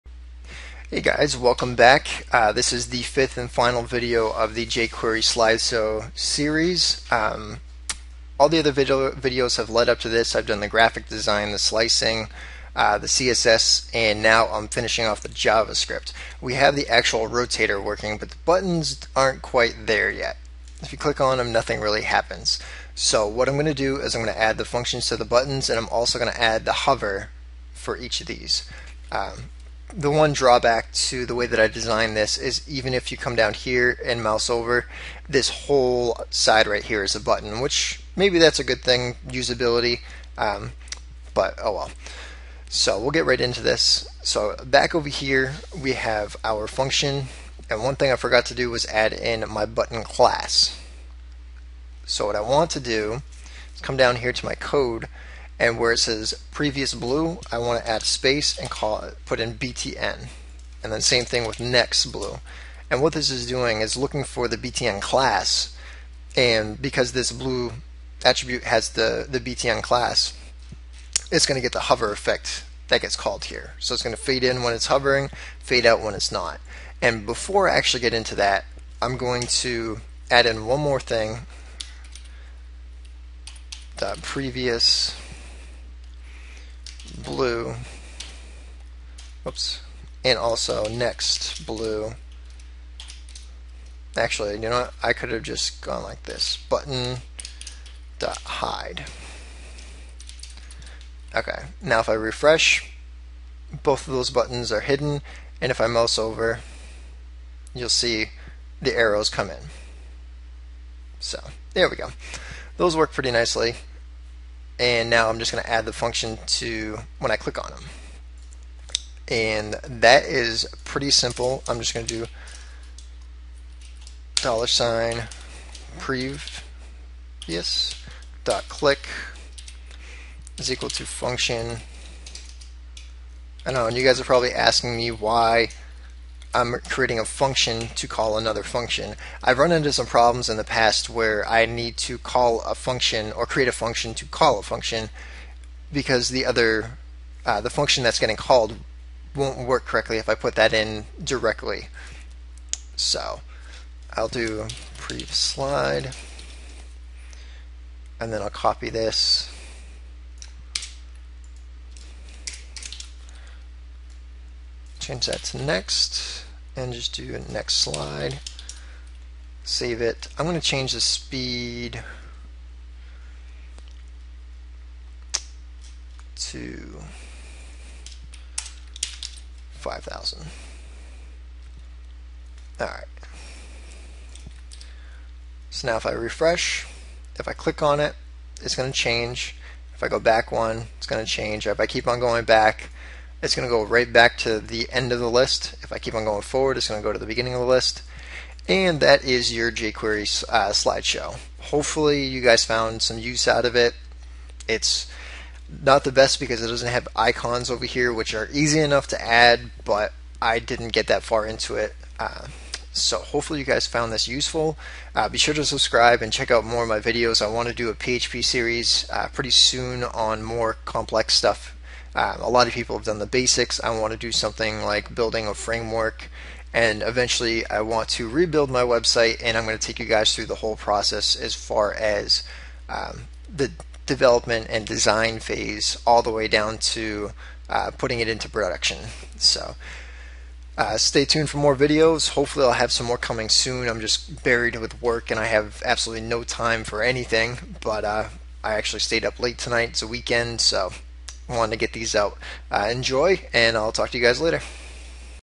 jQuery Slideshow - Part 5
This series of videos will cover creating a layout for your slideshow, and the code behind the slideshow. Jquery will be used for the animations, and everything will be positioned using css.